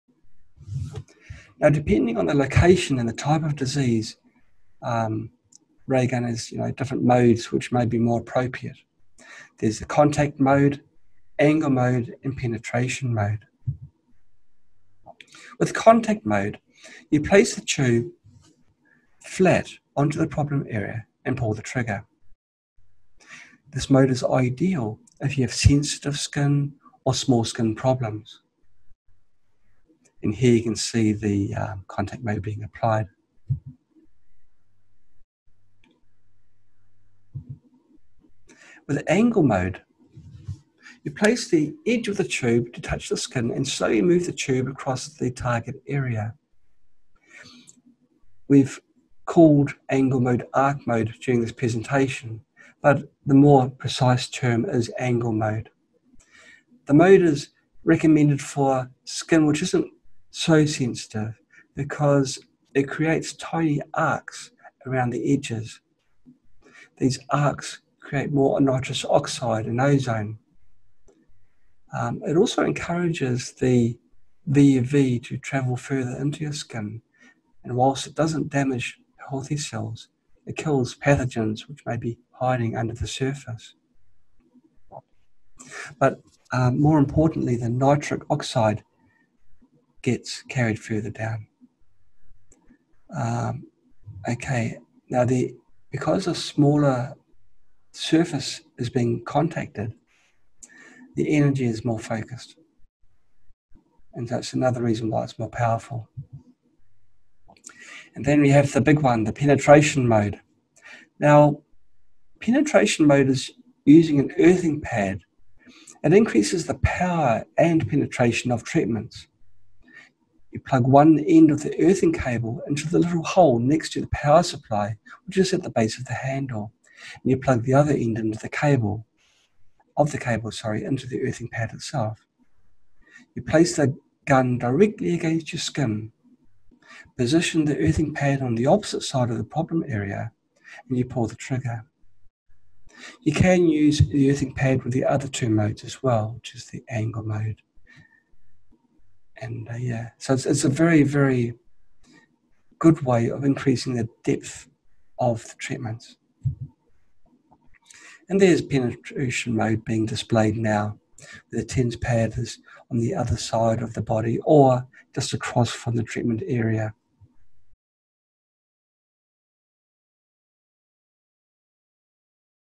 VUV Light Therapy: What Are the Three Modes of MiraMate Ray Gun?
Depending on the location and type of disease, Ray Gun has three modes for you to choose from.

1. Contact Mode
Place the tube directly on the problem area and pull the trigger.

For sensitive skin and small skin problems, it is best to use this mode. The bandwidth of the light is very narrow, with almost no dangerous UV light being produced. VUV light will not harm skin or healthy cells.

2. Angle Mode
Place the edge of the tube to touch the skin, and slowly move the tube across the target area.

This mode is recommended for non-sensitive skin. It provides a higher treatment efficiency because an arc will form around the edges. The arc is a form of cold plasma which creates more nitrous oxide and ozone. This mode has higher power than contact mode because the energy is more focused.

3. Penetration Mode
This mode uses an earthing pad to increase the power and penetration of treatments. Plug one end of the earthing cable into the earthing port. Plug another end of the cable into the earthing pad.

Place the gun tube directly against the skin. Position the earthing pad on the opposite side of the problem area.

This mode can be used in combination with the other two modes.